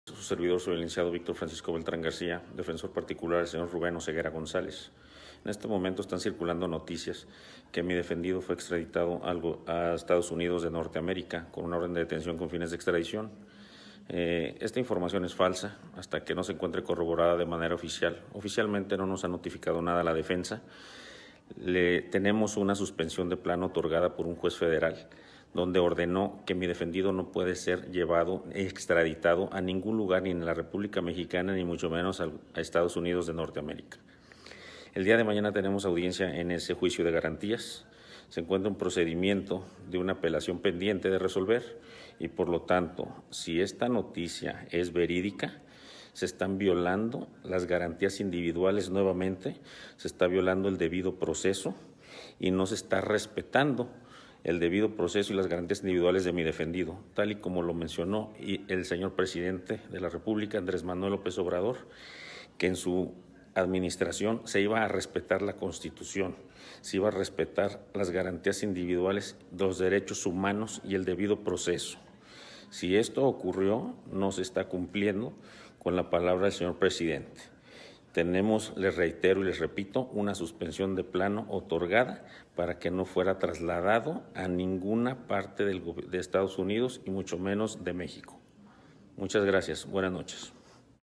EfectoTV | Abogado de "El Menchito" niega que su cliente haya sido extraditado a EUA
VIDEO | Abogado de "El Menchito" niega que su cliente haya sido extraditado a EUA

EfektoTV está basado en sólidas propuestas de contenido informativo objetivo, plural, independiente y analítico.